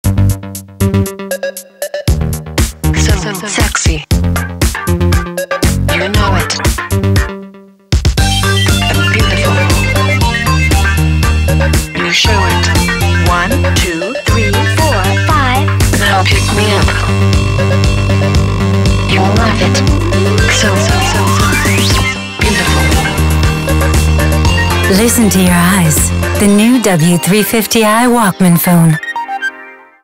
Parasol Island SONY ERICSSON
Sometimes a good idea just needs a simple execution. Sometimes not. For the UK market we hitch a ride with Sony's new mobile gems and parasol animation director Steve Scott to travel through some very power poppin' music sleevery.